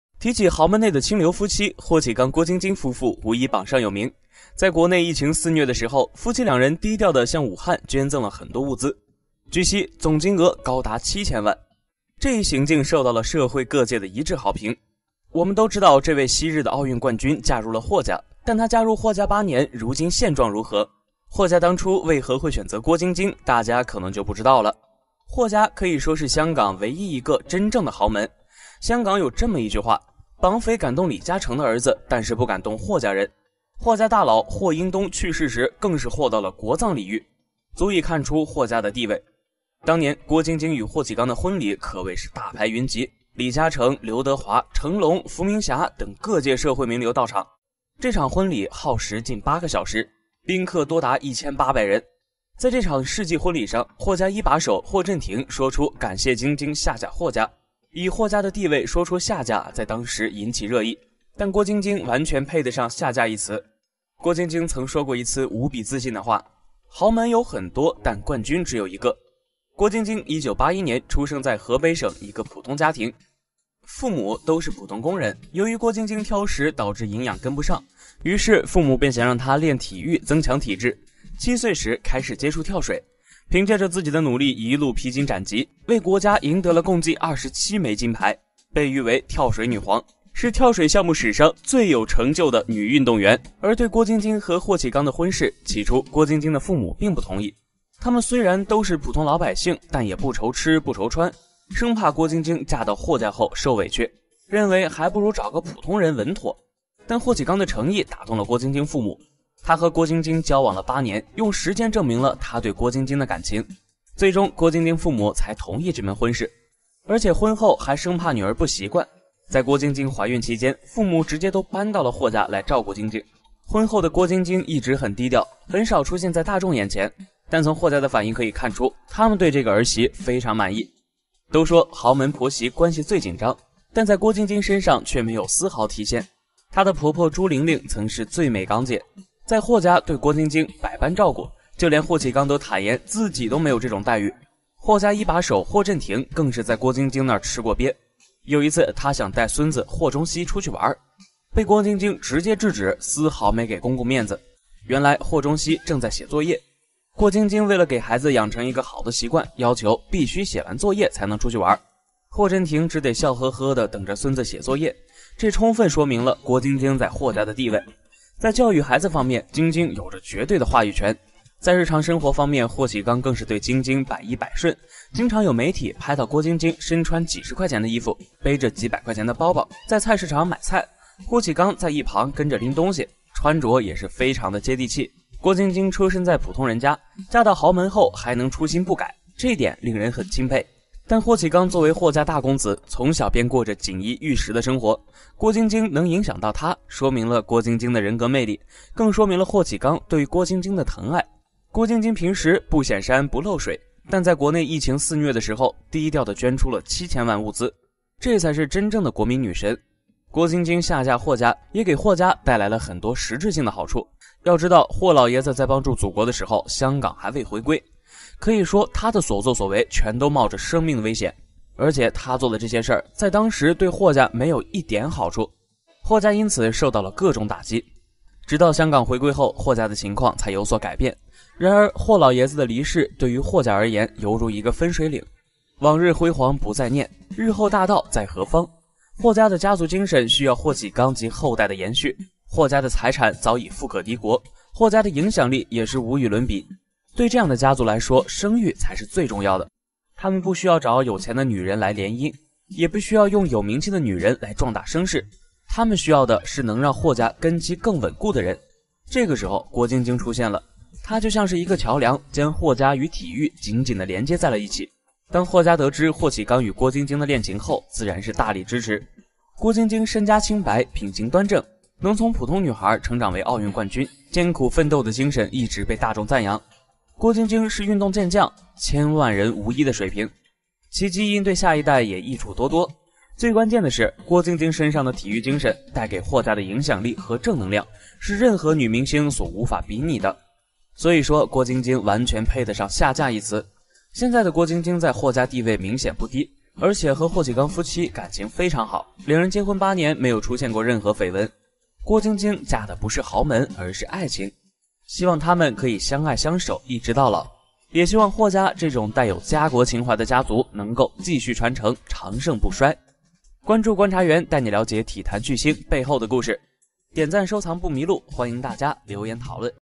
跳水女皇郭晶晶：为何下嫁豪门地位不减？冠军身份秒杀所有女星！【体娱一品官】
体育 跳水 郭晶晶 豪门 我们的内容专注于体育赛事，绝对不会包含任何暴力或血腥元素。